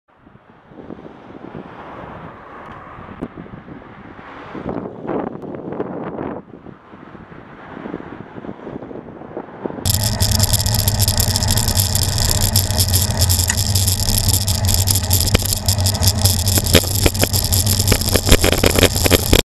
FL53OJG pinch point
Look, stick out arm, move to the right hand side of the lane, surely no one will try and undertake at the pinch point?

Thursday 4th September 2014 approx 08:25